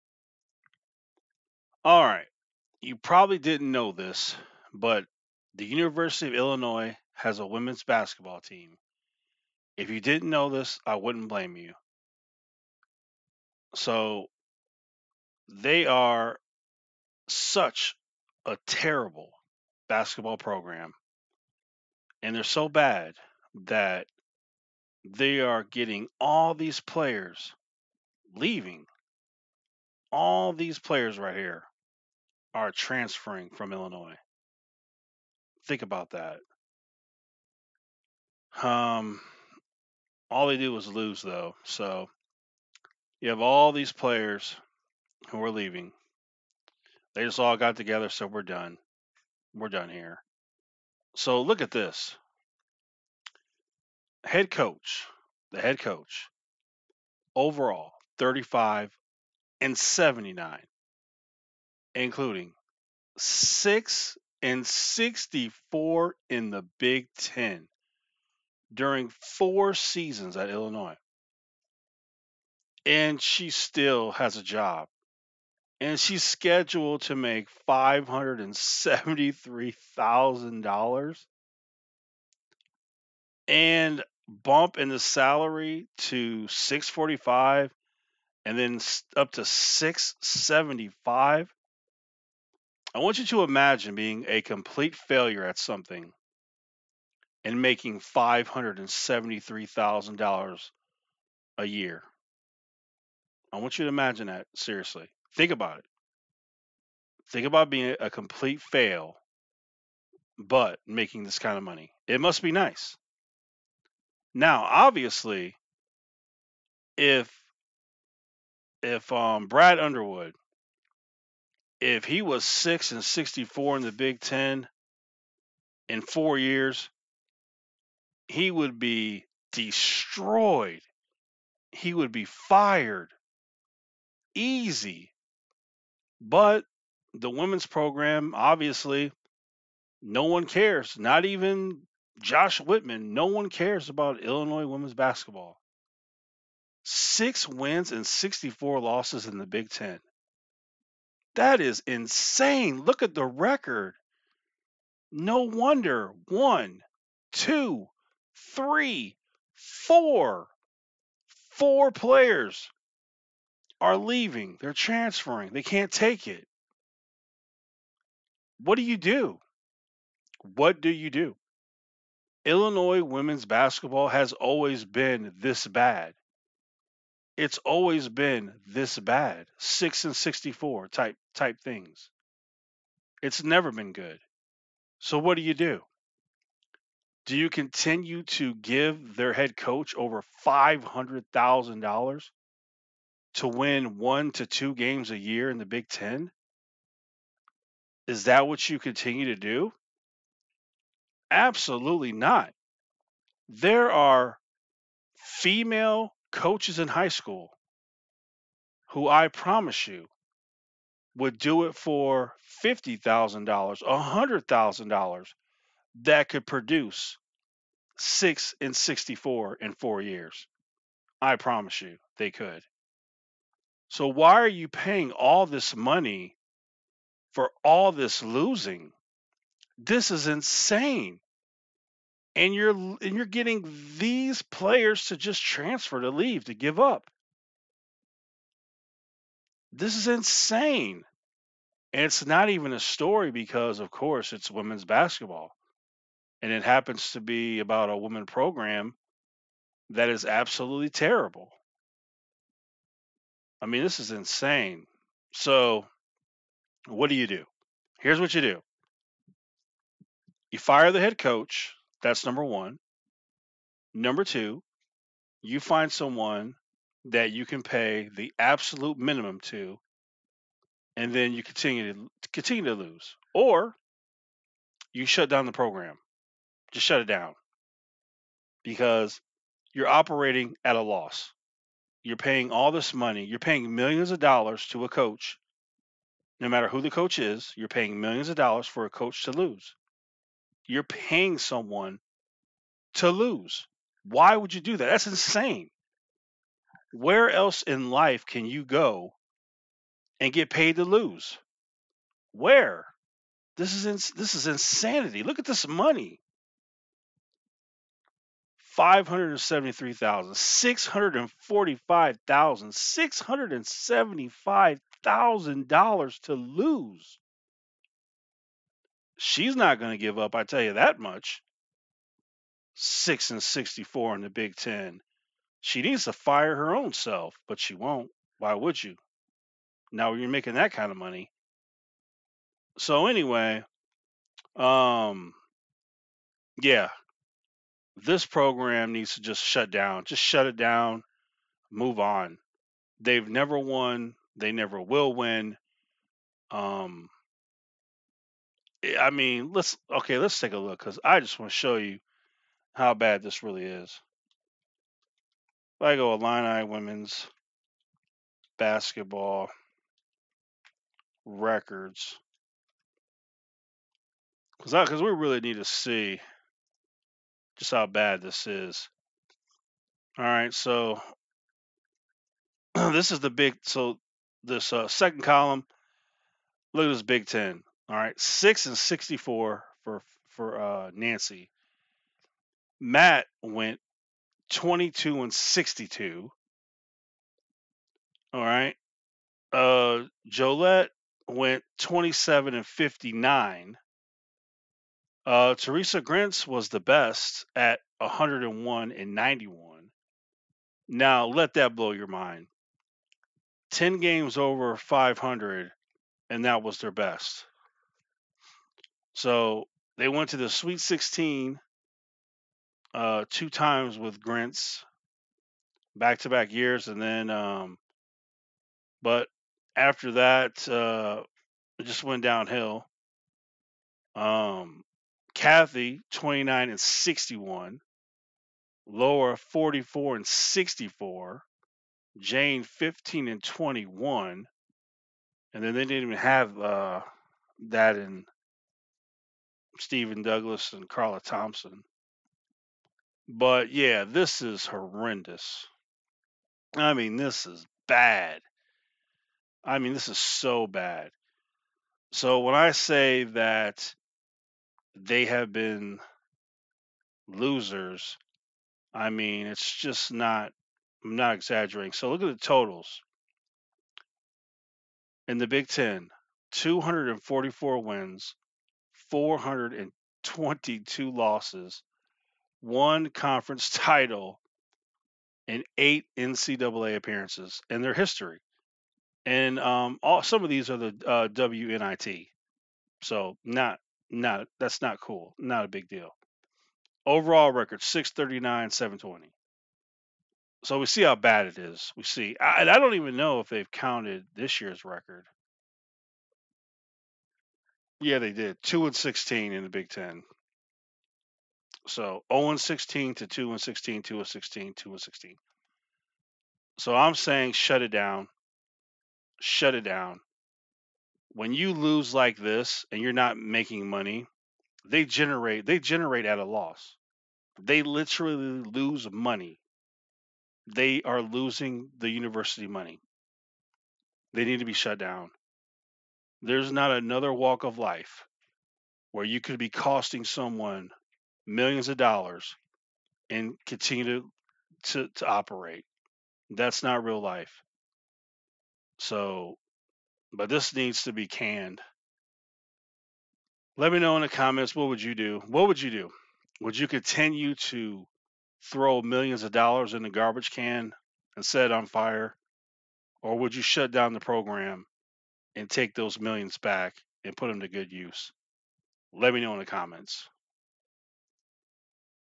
Illinois Women's Basketball Needs to End!
Illinois Women's Basketball Needs to End!  All they do is lose players and games!